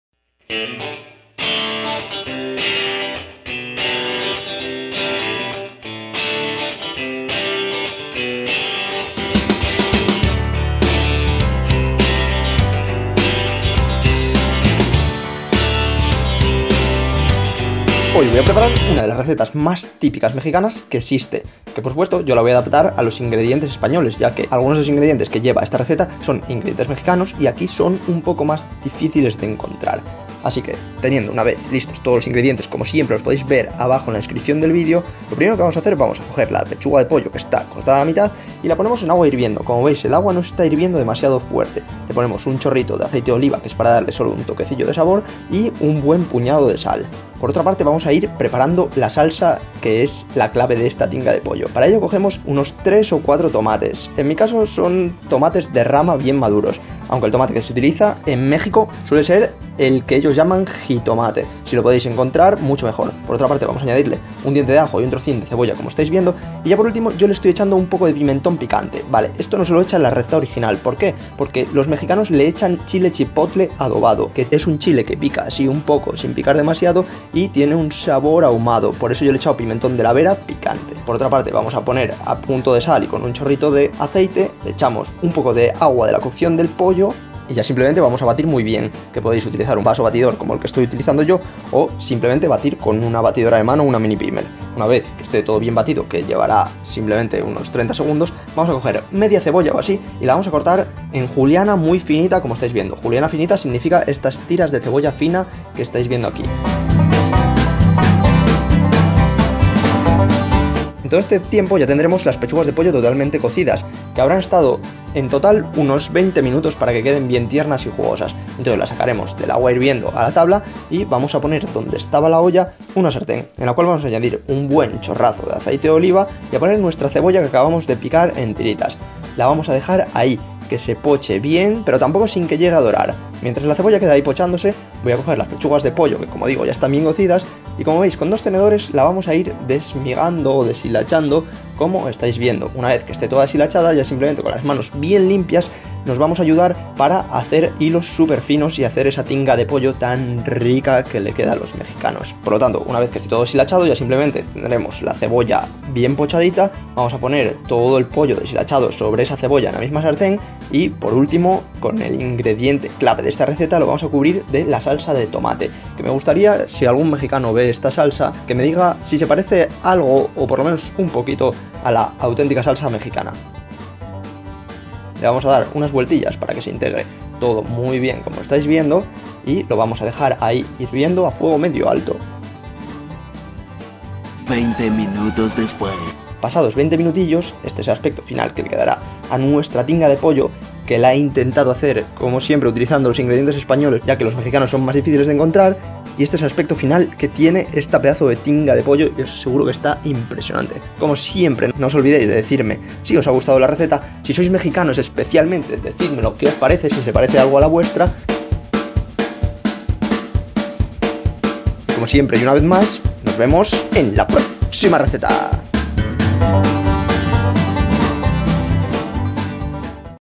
Cómo hacer TINGA DE POLLO Mexicana
Cómo hacer TINGA DE POLLO Mexicana. Aprende a hacer la receta de tinga de pollo o pollo deshilachado con salsa mexicano.